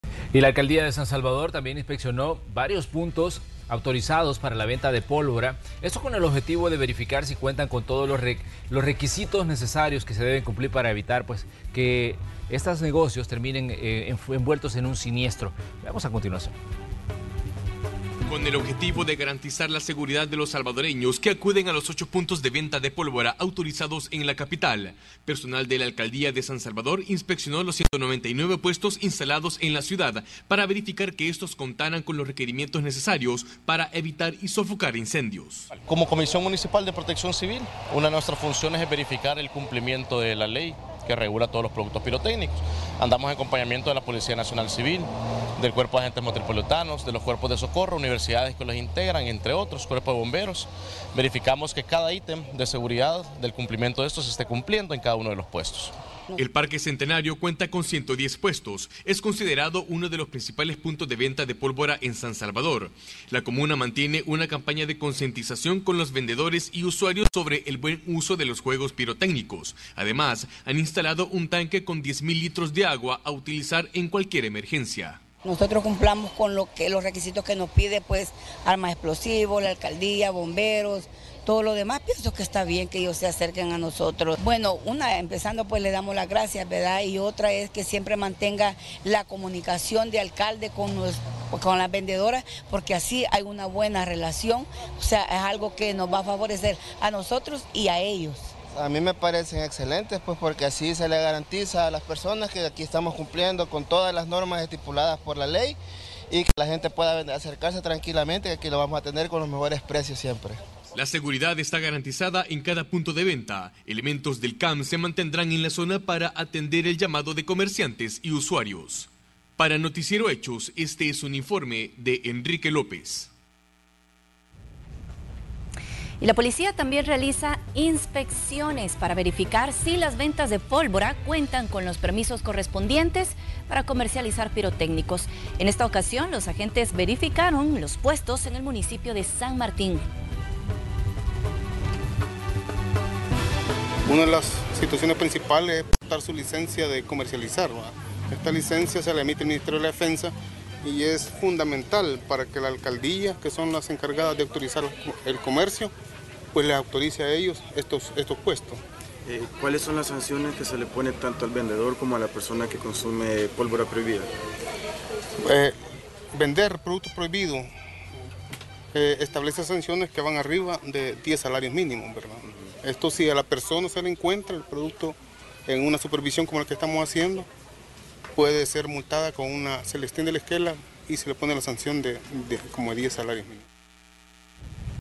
Alcaldía de San Salvador inspeccionó establecimientos de venta de pólvora
La alcaldía de San Salvador inspeccionó varios puntos autorizados para la venta de pólvora, esto con el objetivo de verificar su cuentan con los requisitos necesarios para evitar siniestros.